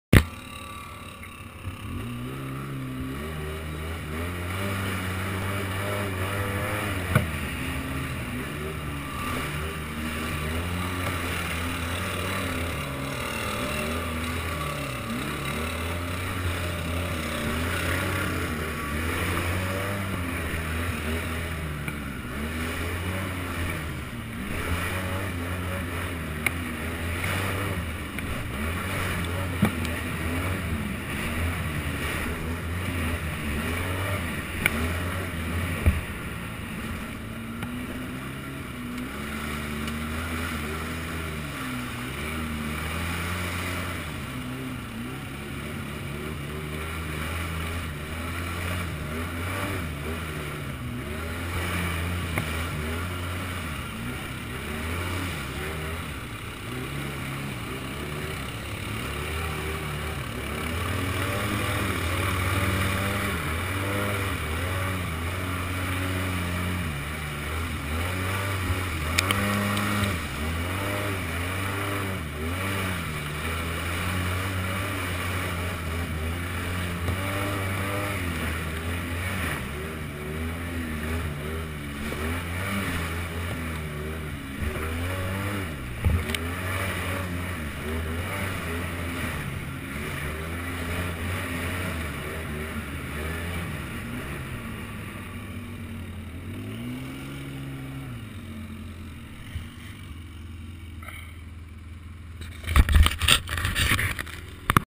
Polaris Sportsman smashing the famous B-Trail
A nice day with some bud's out on the trail - a lot of fun and mud...lol!